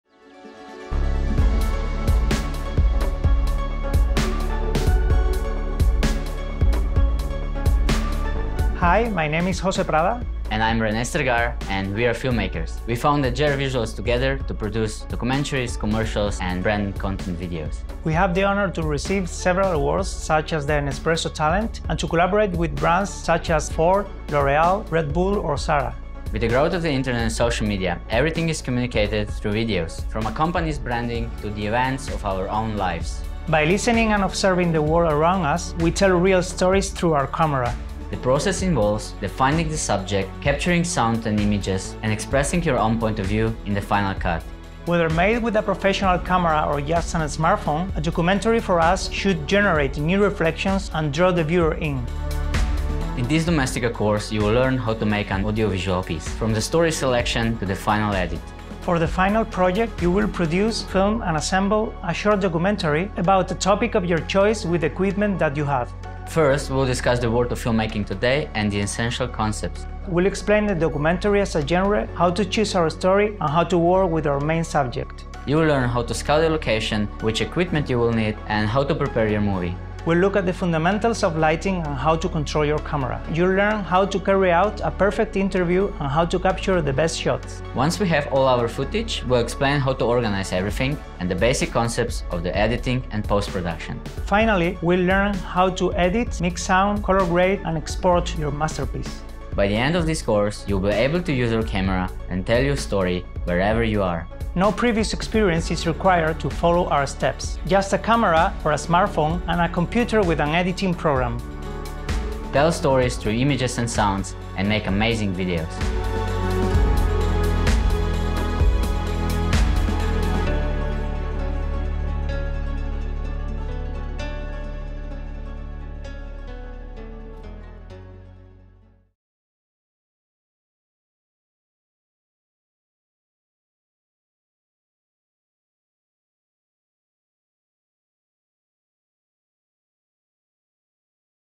CURSO ONLINE Creación de un documental desde cero de Jose Prada & Rene Strgar
Aprende a desarrollar una pieza audiovisual completa para comunicar tu historia, desde la idea inicial hasta la postproducción